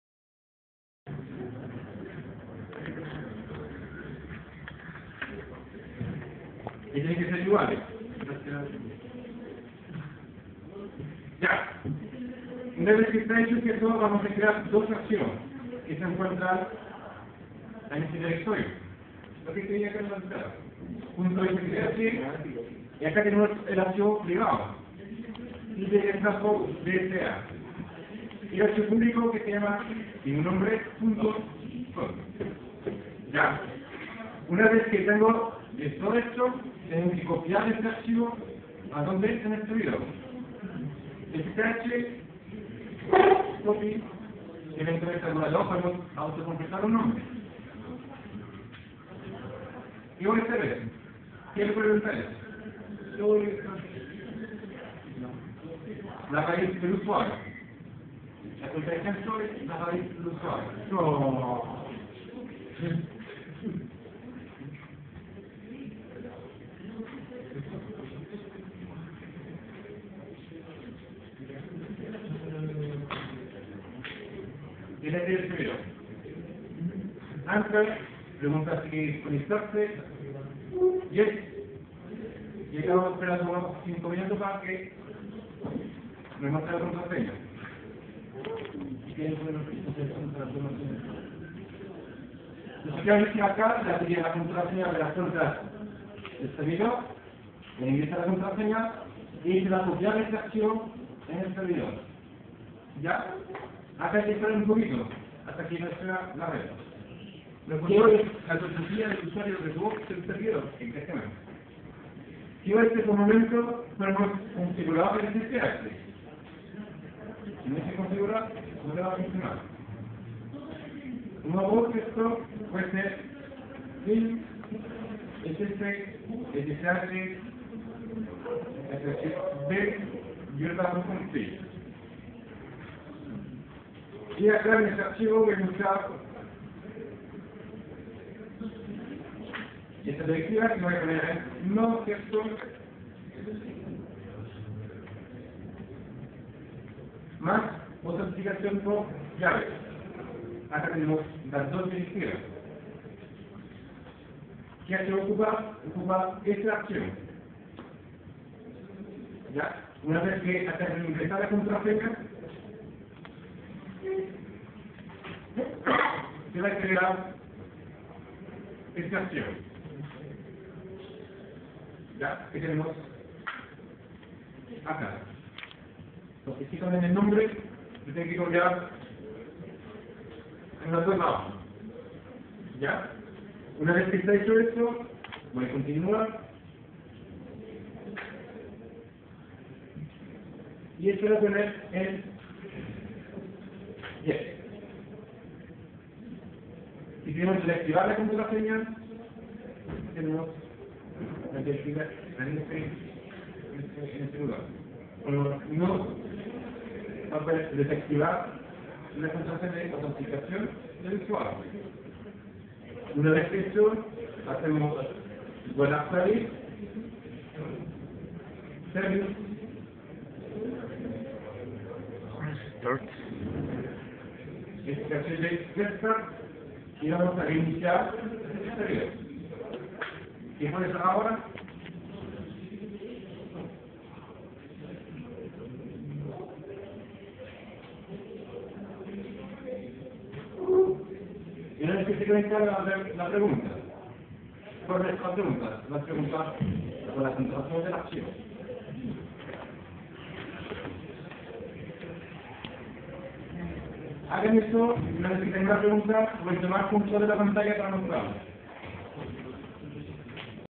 SSH LInux Key DSA Public
Video en Clase de Matthias Hablando Sobre Como crear una Llave Privada en SSH